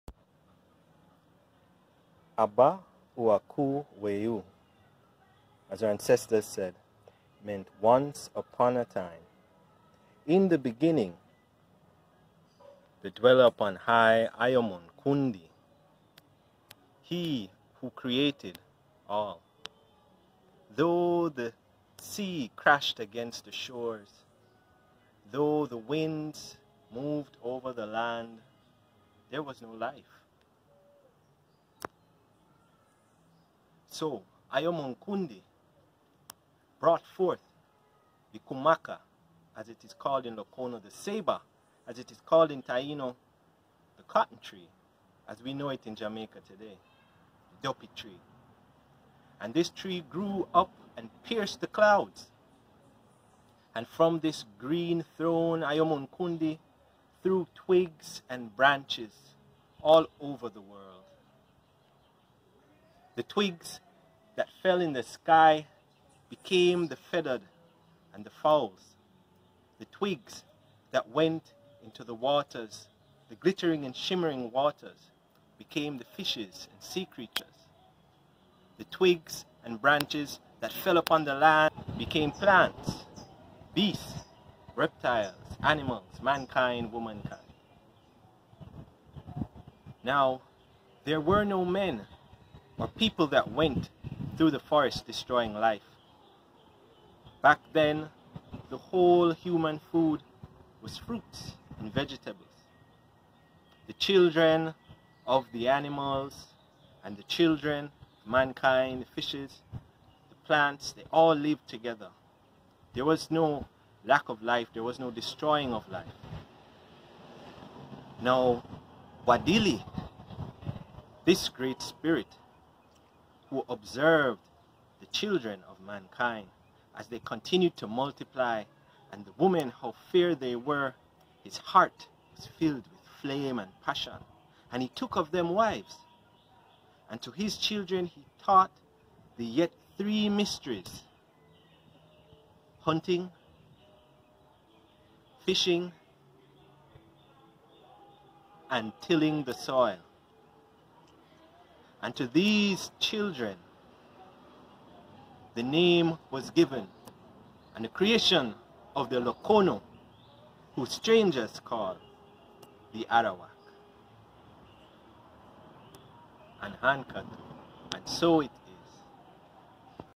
Arawak Creation Story
Arawak Creation story that was share at Ananse Soundsplash 2021